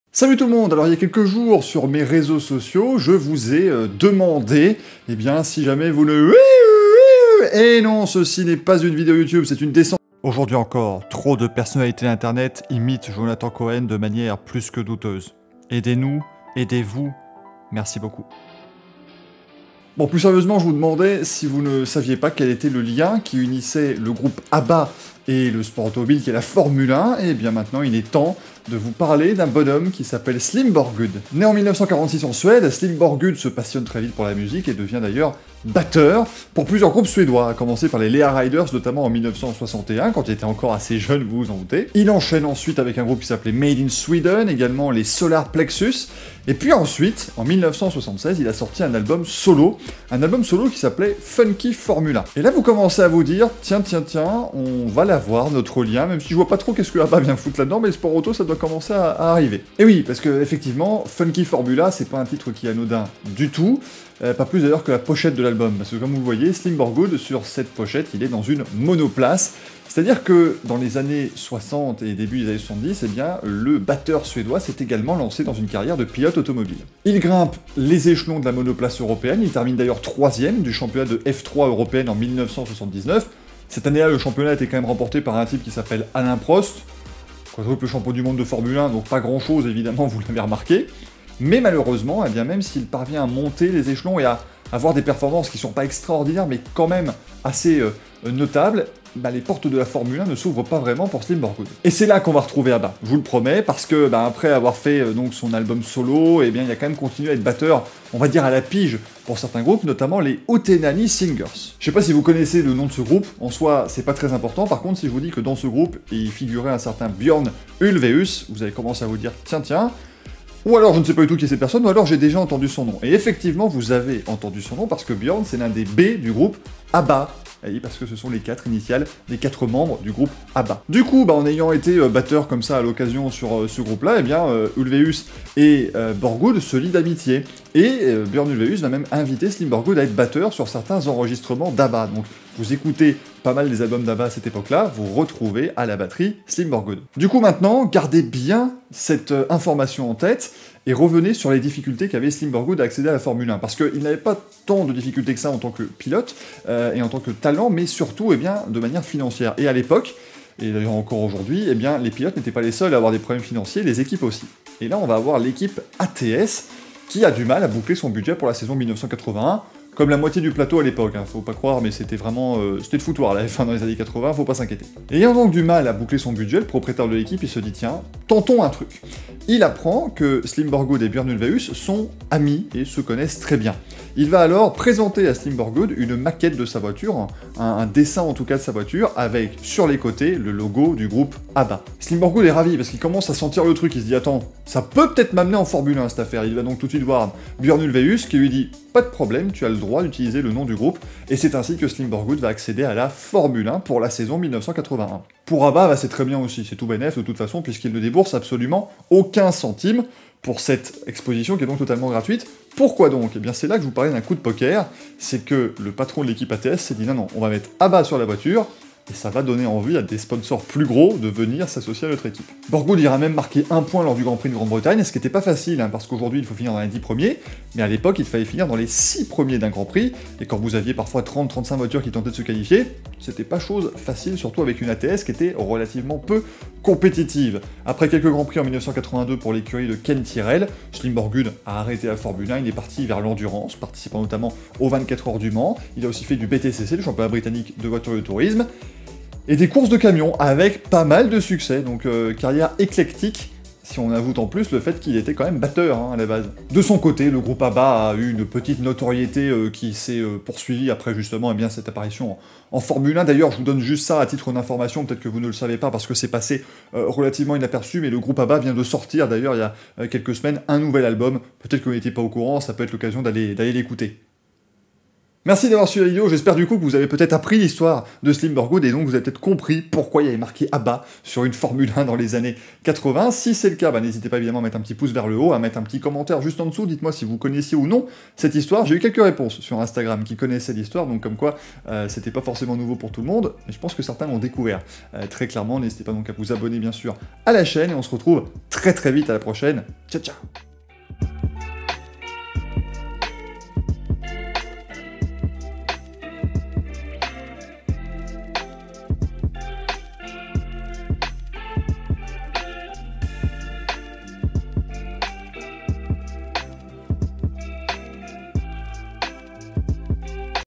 Slim Borgudd, le batteur d'ABBA devenu pilote de F1 !
Il y a comme ça des liens qu'on ne s'attend pas à faire dans la vie, comme trouver un lien entre ABBA et la Formule 1 via le batteur suédois Slim Borgudd ! Je vous raconte son histoire...